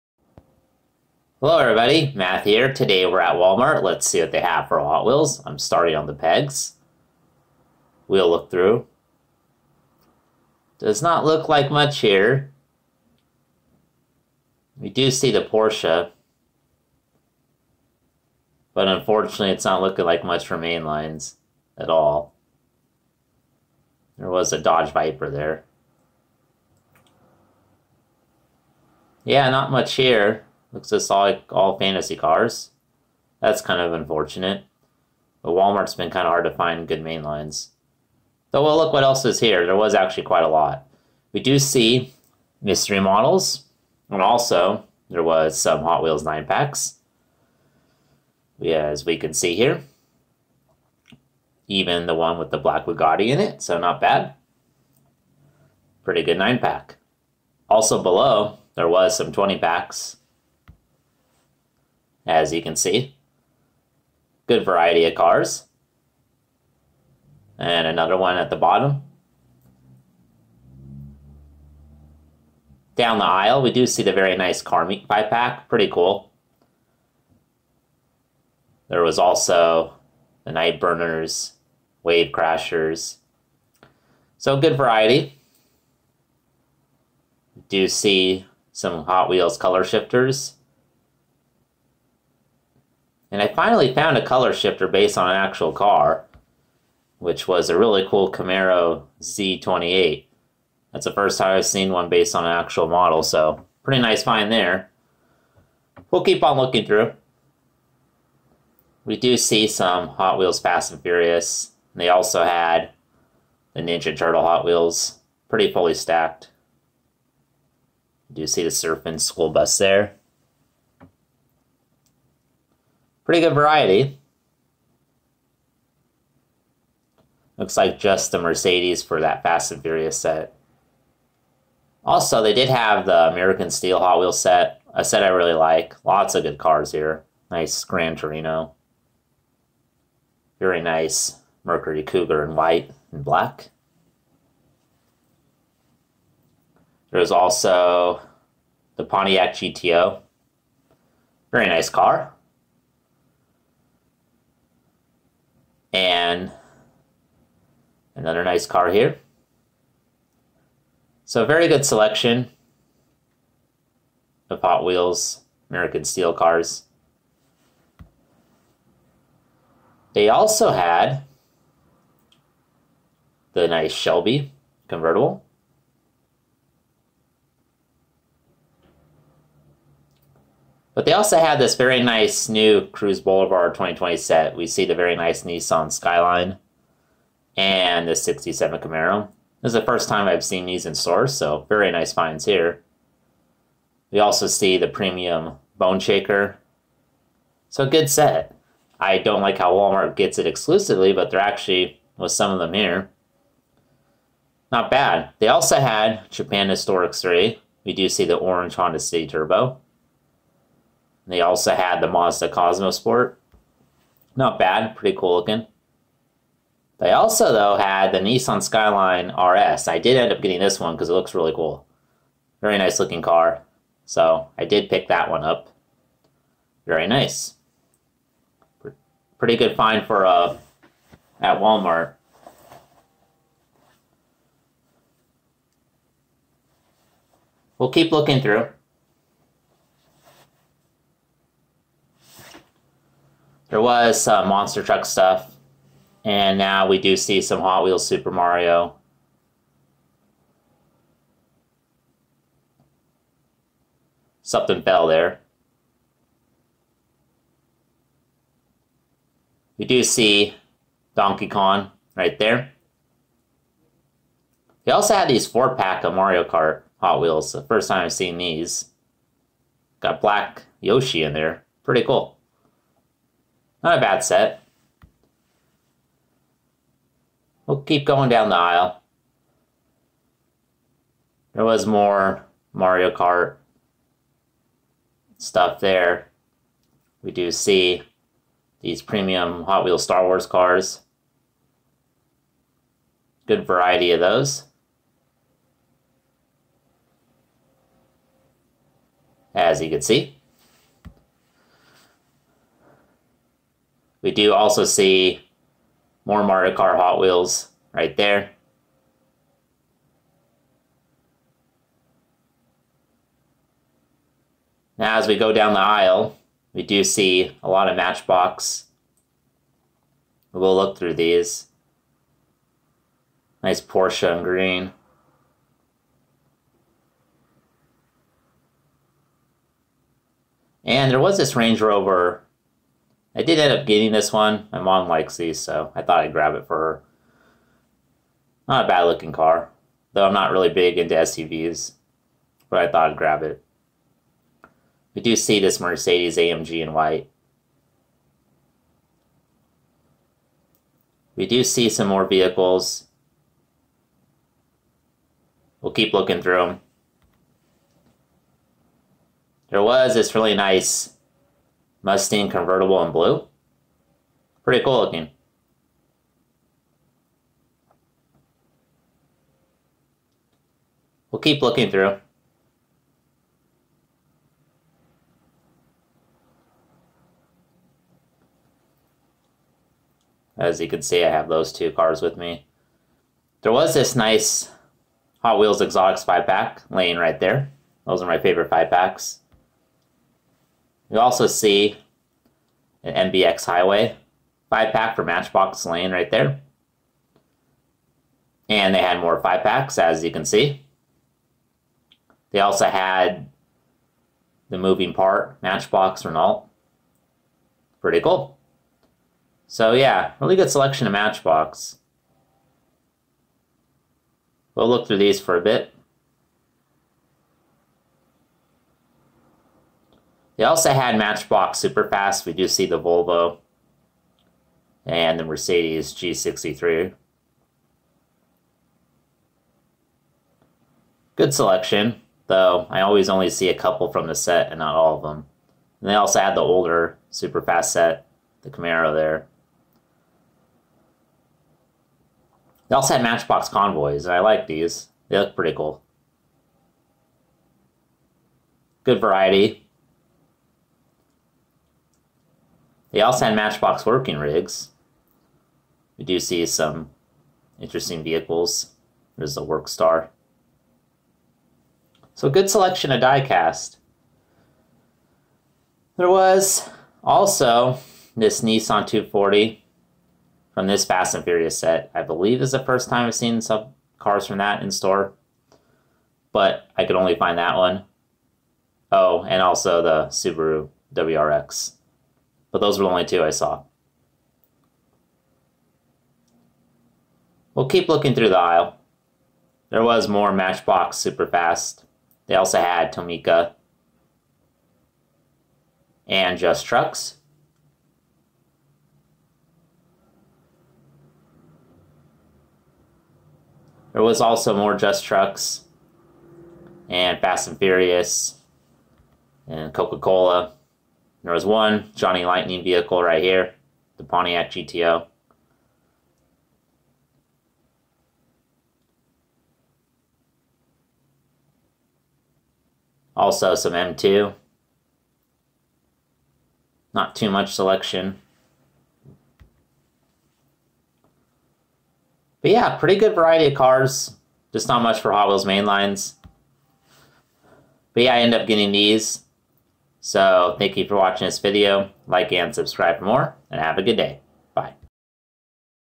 Peg Hunting Hot Wheels At Walmart Episode 37! New 2020 Cruise Boulevard Cars In Stock!
Another peg hunting video at a local Walmart. Some good finds for Hot Wheels! The main highlights was that Walmart had some of the new 2020 Cruise Boulevard in stock! This included the Nissan Skyline GT-R! There was also a good variety of Hot Wheels 5 Packs, 9 Packs, and 20 Packs!

For other Hot Wheels, there was a good variety of different Hot Wheels sets! These included Hot Wheels American Steel, Hot Wheels Fast  And Furious, Hot Wheels Color Shifters, Hot Wheels Japan Historics 3, and Hot Wheels Mystery Models! It was a really good selection. The only thing that was lacking was good Hot Wheels mainlines.

For other diecast, there was a good variety. There was a decent selection of Matchbox mainlines, along with a good selection of 5 Packs. There was also Matchbox Superfast and Matcbox Convoys in stock. Finally, there was some Tomica, M2, and Johnny Lightning.

I ended up getting the Nissan Skyline RS and the 2014 Range Rover Evoque.

If you like this peg hunting video, check out my peg hunting playlists!